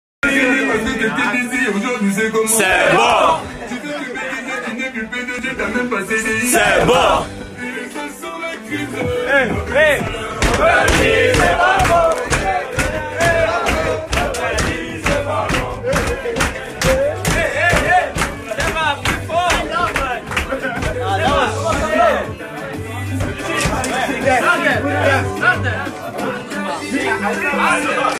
Behind the scenes: RSC Anderlecht home
Enjoy these exclusive dressing room images after the fantastic 3-0 victory against RSC Anderlecht on 30/03/2019!